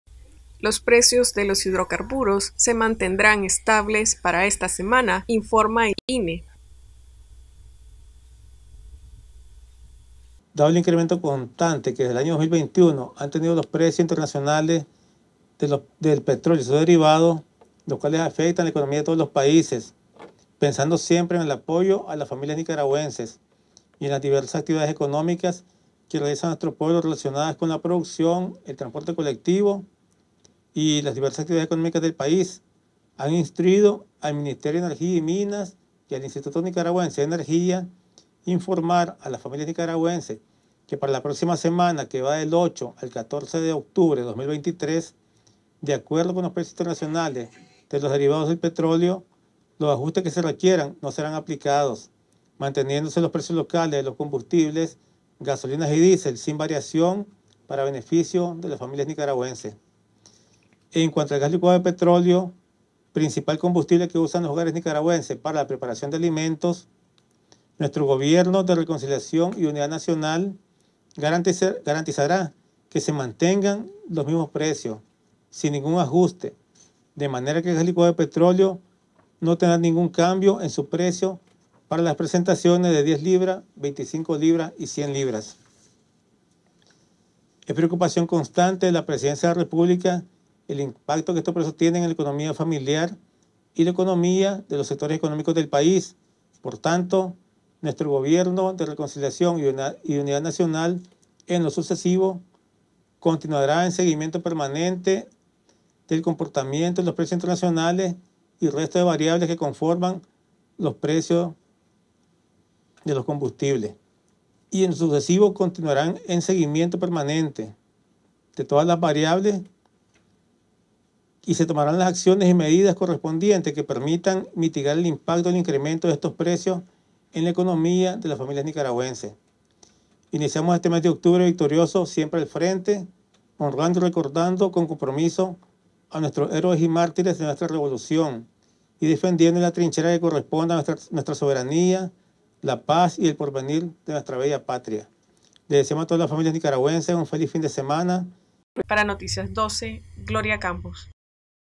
Noticias12Nicaragua Precios de los Combustibles se mantienen congelados informa INE
Precios de los Combustibles se mantienen congelados informa INE